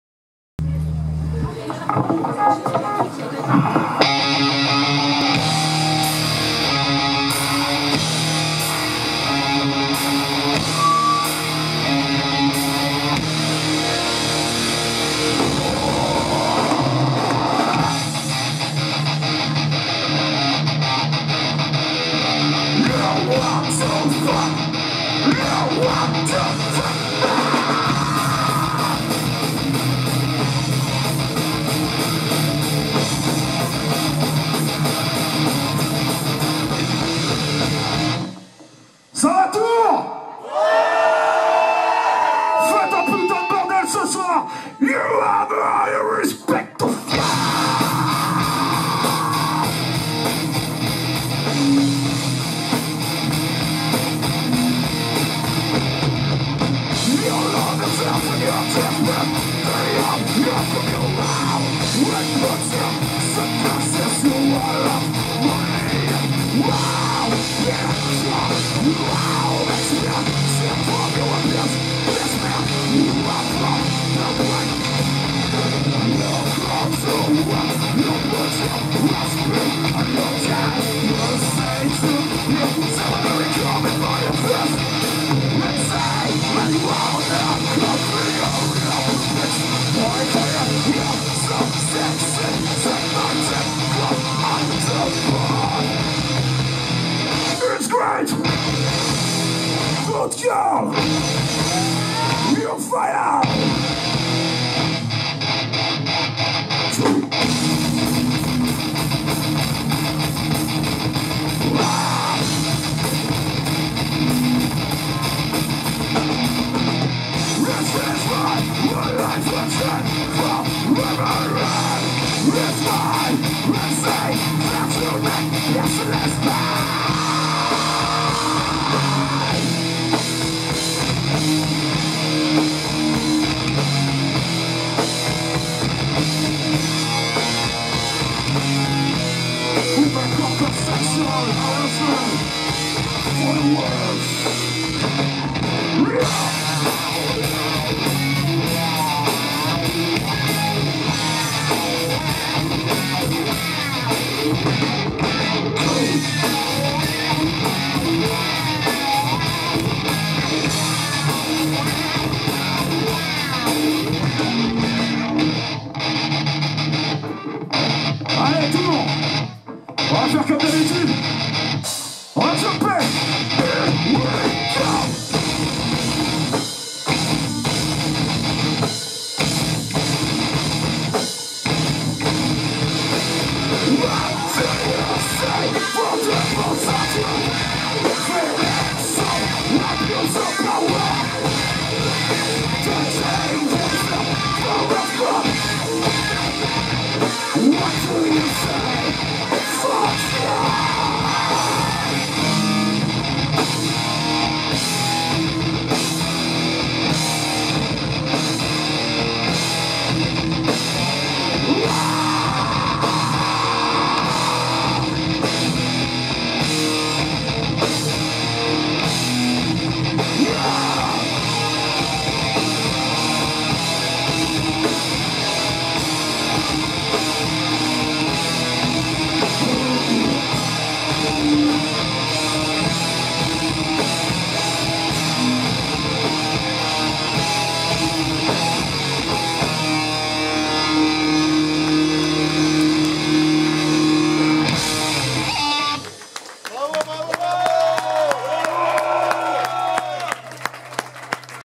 Born to Burn live le 13/05/17 pour le Sid festival à Tours
(Metal HardCore de Tours)
Concert organisé par Rock Style !!!
"Centre Socioculturel Léo Lagrange Gentiana"